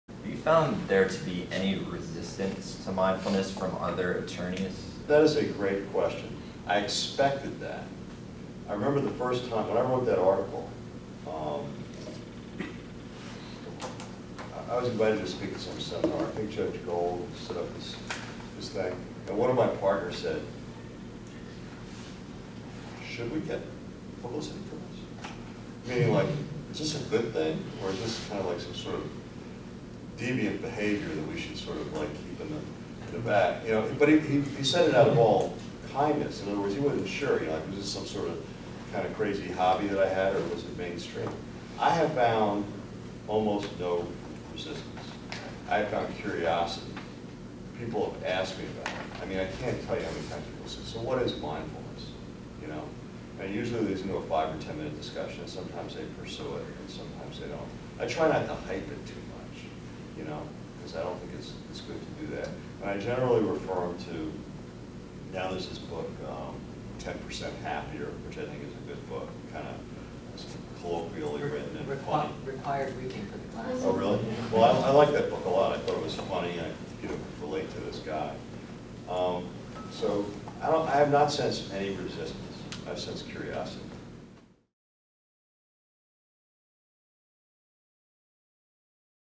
Harley Tropin at Miami Law- Lawyer Reactions to Mindfulness
This video is an excerpt from Professor Scott Rogers' Mindfulness in Law Class at the University of Miami School of Law. In this clip, Harley Tropin, a Miami attorney and founding partner of Kozyak, Tropin & Throckmorton, discusses lawyer reactions to his mindfulness practice.